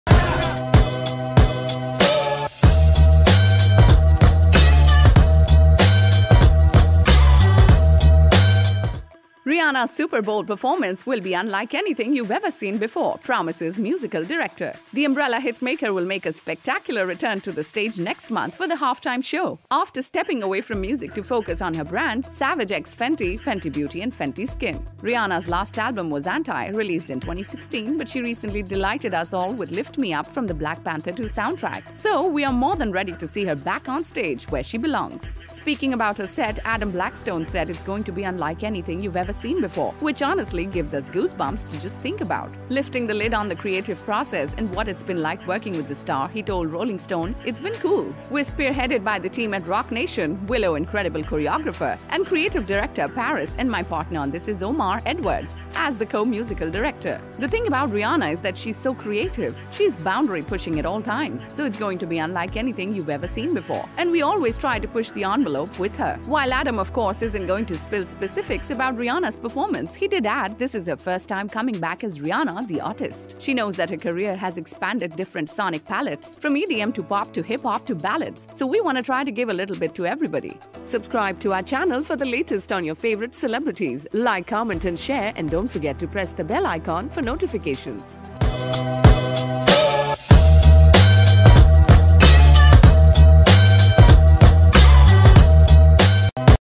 Rihanna’s Super Bowl performance "you’ve ever seen before" promises musical director
Rihanna’s musical director has been dropping hints about her upcoming Super Bowl show and we are bursting with excitement.